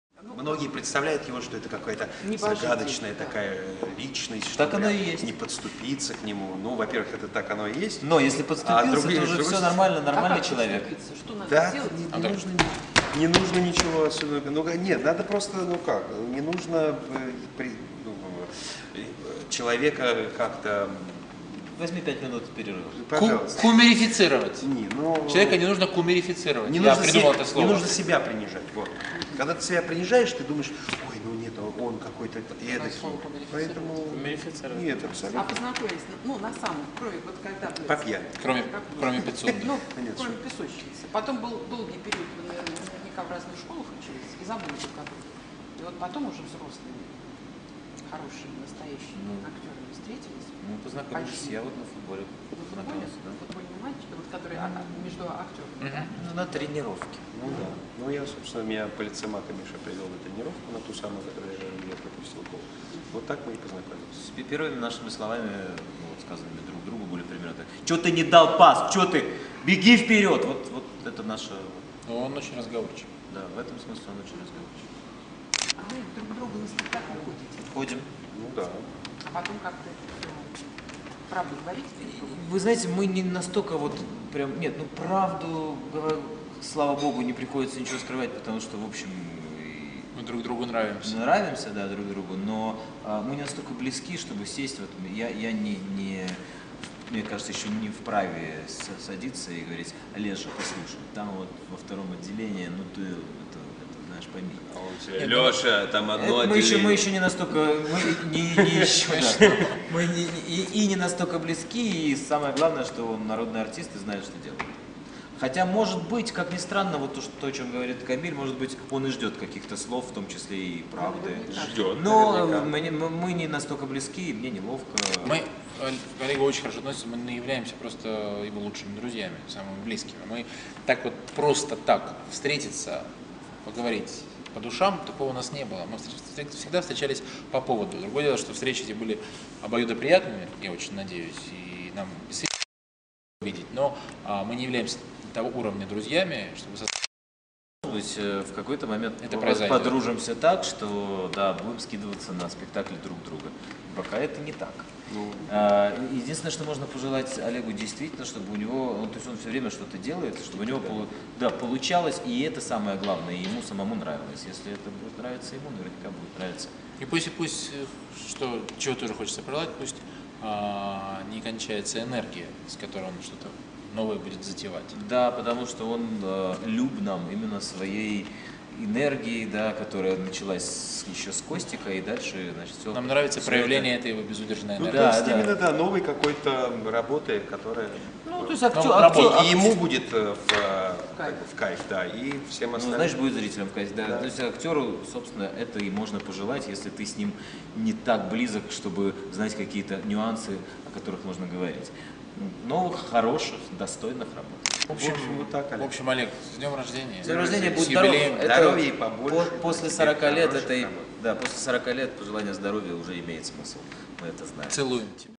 Юбилей Олега Меньшикова 10
Поздравление квартета "И", ролик газеты "Комсомольская правда".